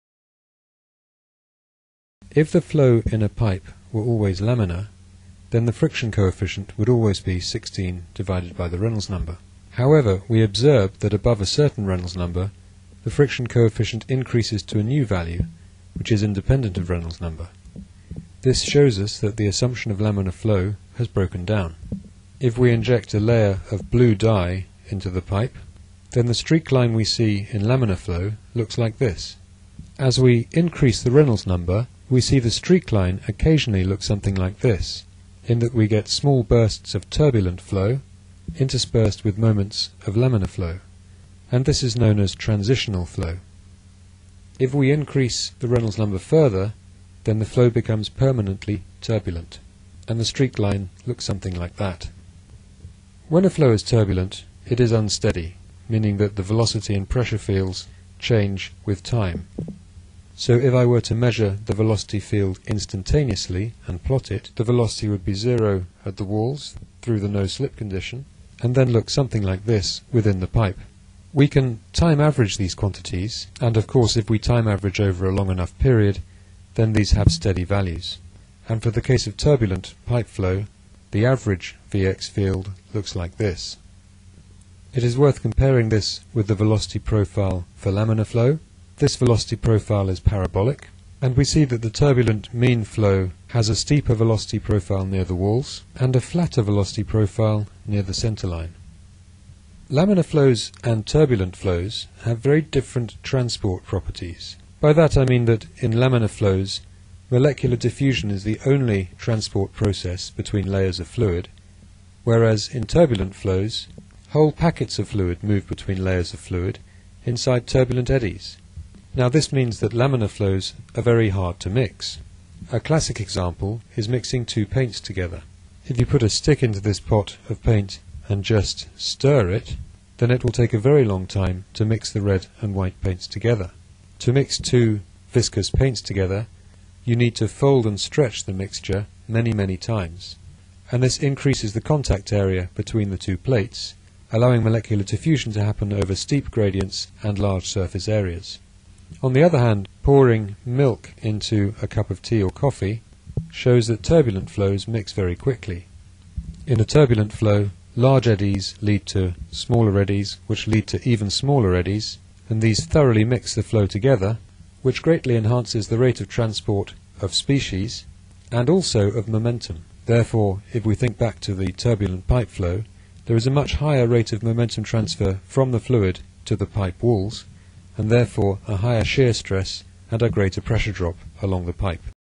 5.3 Laminar flow, turbulent flow, and mixing
At high Reynolds numbers, flows become turbulent. Turbulent flows rapidly mix fluids, while laminar flows do not.